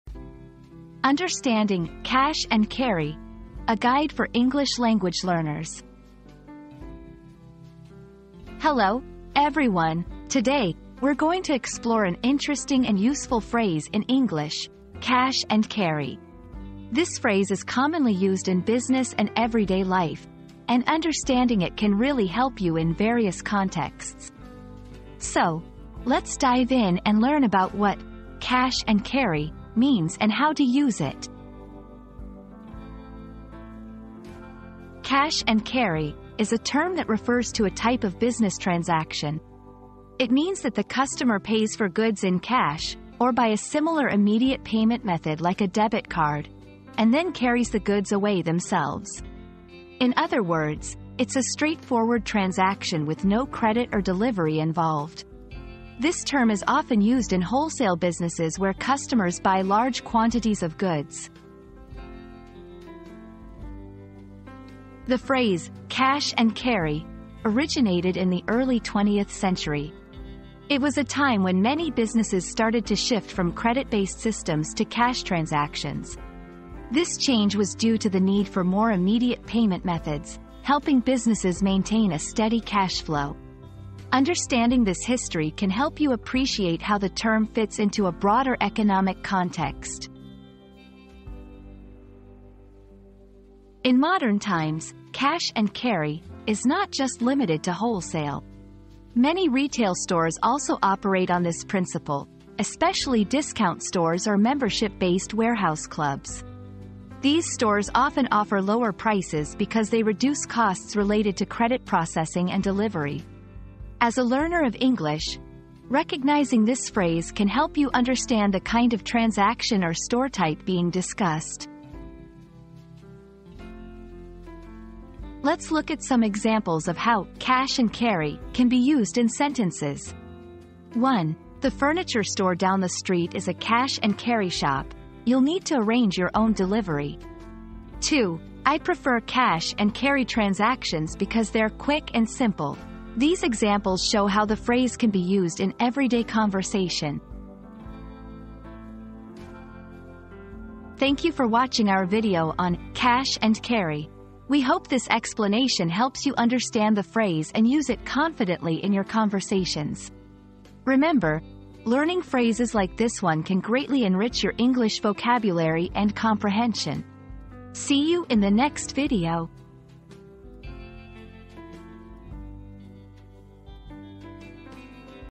Understanding "Cash and Carry": A Guide for English Language Learners
00:00 • Introduction - Understanding "Cash and Carry": A Guide for English Language Learners
00:34 • The Meaning of "Cash and Carry"
01:04 • Origin of the Phrase
01:33 • Cash and Carry in Modern Times
02:04 • Usage in Sentences